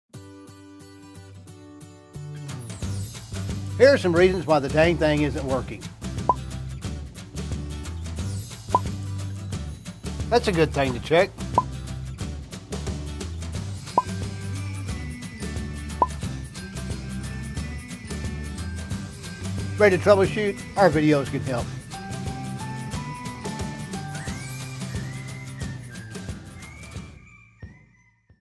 Top reasons why - Refrigerator won’t cool
Here are the top 5 reasons your refrigerator won’t cool. Need some help troubleshooting? Our videos can help.